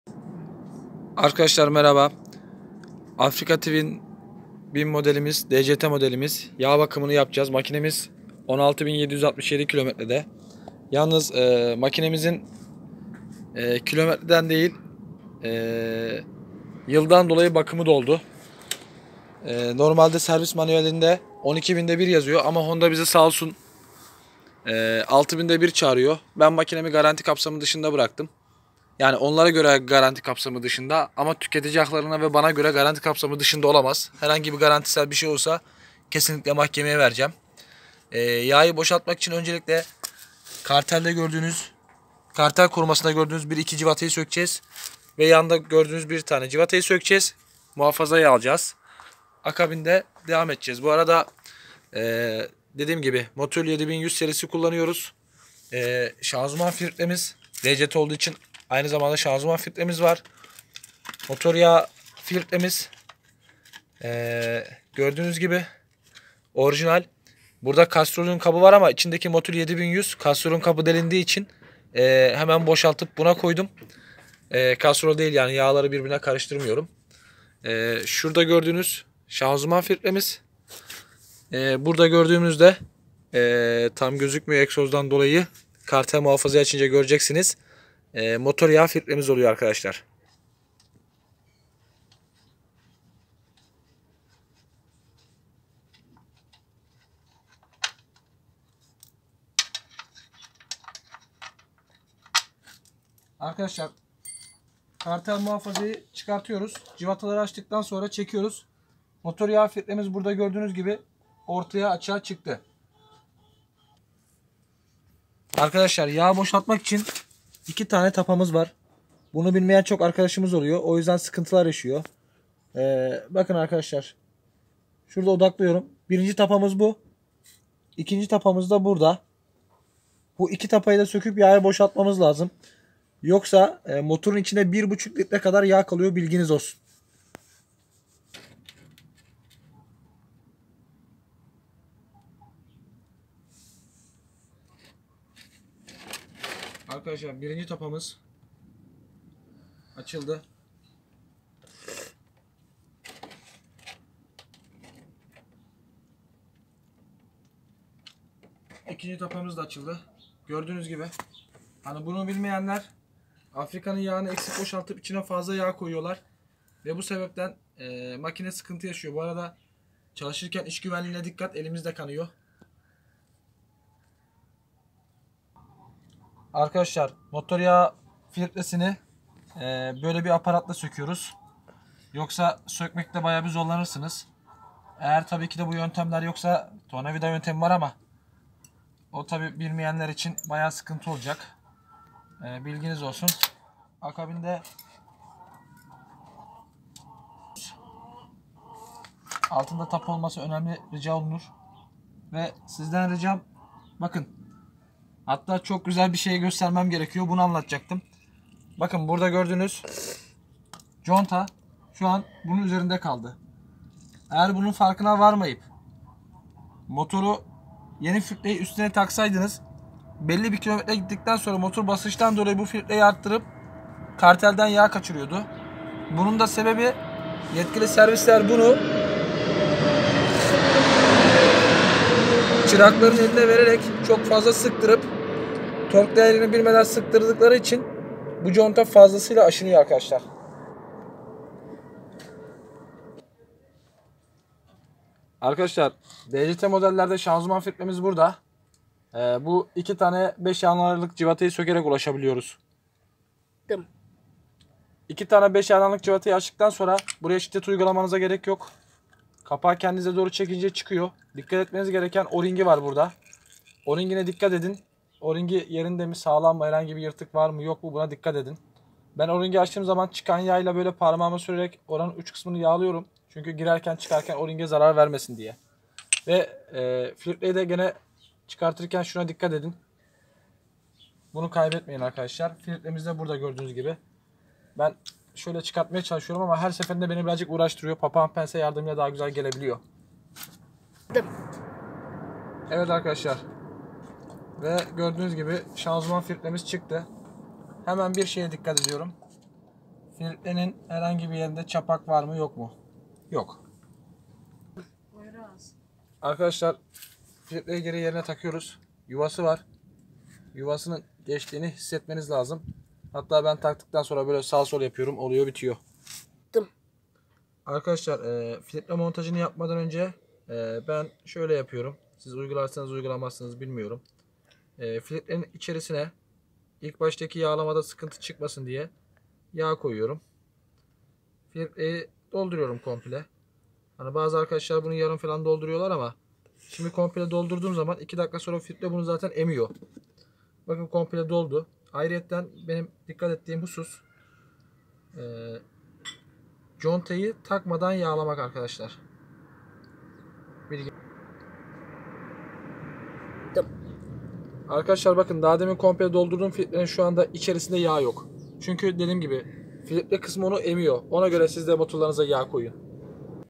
Honda Africa Twin Yağ ve filitre Değişimi
Honda Africa Twin dct  Yağ ve filitre Değişimi.
Yağ filitresi
Şanzuman filitresi
Motor yağı
Motul 7100